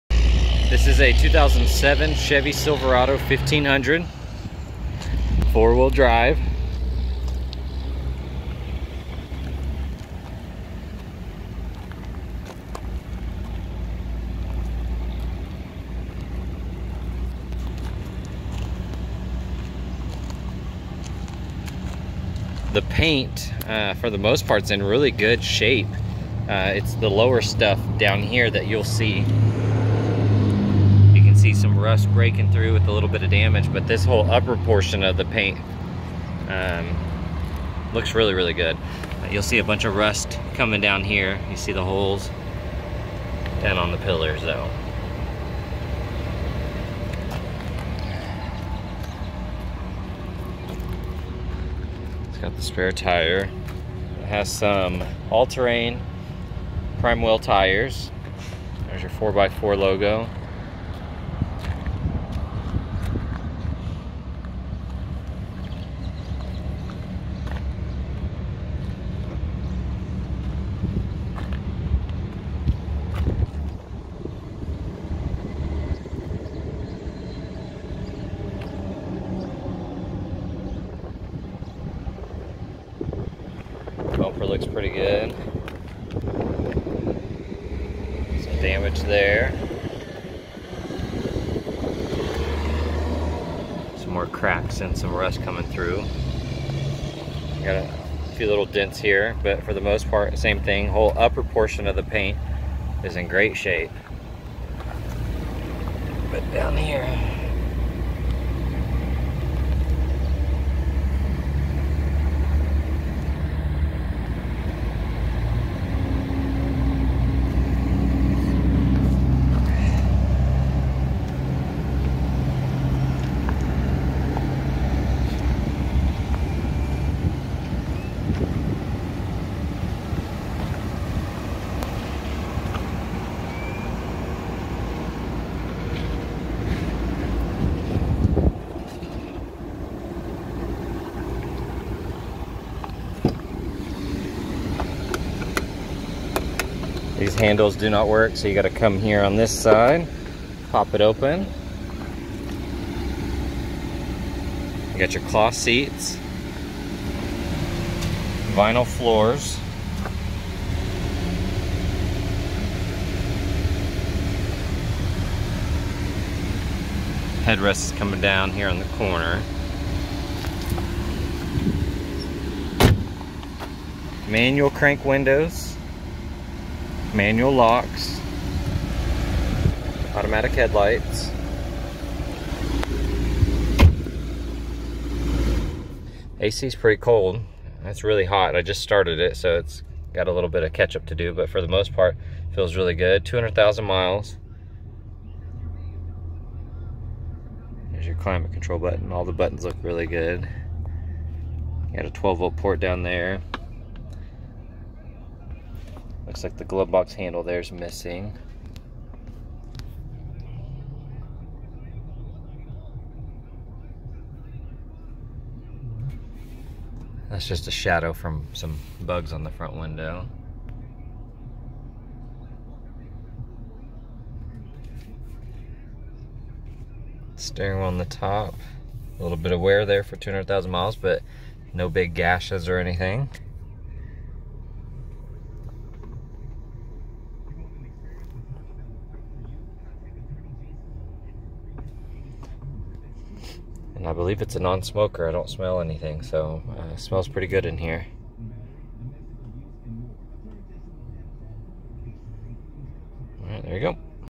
2007 Chevy Silverado Work Truck For Sale - Stock # 23274A
CLASSIC WORK TRUCK * 4WD * 5.3L V8 * 4-SPD AUTO * DOUBLE CAB * 200K MILES * DARK CHARCOAL CLOTH SEATS * GRAYSTONE METALLIC PAINT * AM/FM/CD * LOCKING REAR AXLE DIFF * HD TRAILERING PKG. * OFF-ROAD SKID PLATE * VINYL FLOORS * MANUAL WINDOWS, LOCKS AND SIDE MIRRORS * AUTO ON/OFF HEADLIGHTS AND MORE!